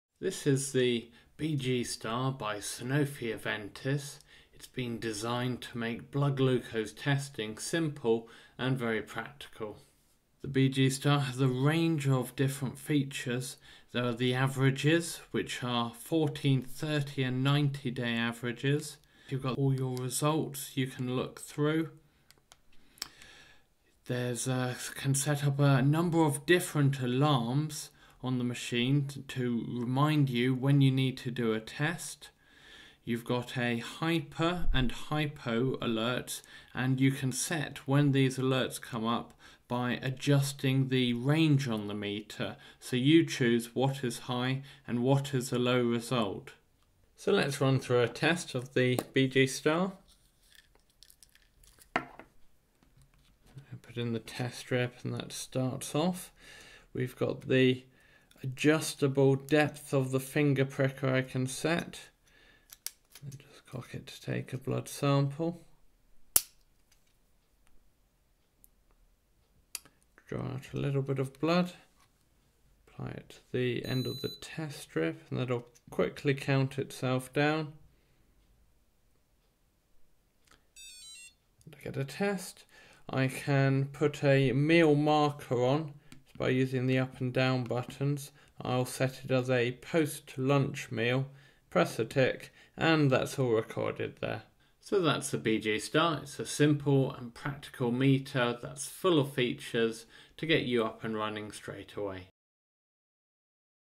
BGStar blood glucose meter review
The BGStar is an intuitive to use meter with a design that won't look out of place next to a mobile phone - with it's sister meter, the iBGStar plugging into an iPhone to give your blood glucose test results.